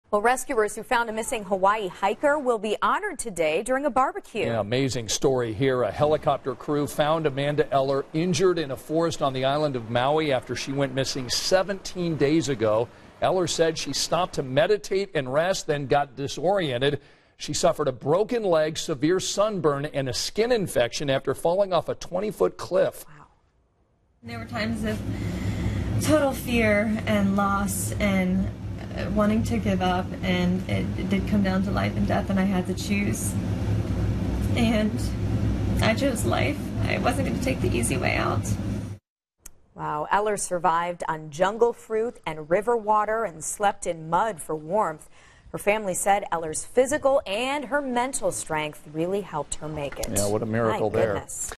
Hiker rescuers to be honored
The three volunteer rescuers who found a missing Hawaii hiker will be honored.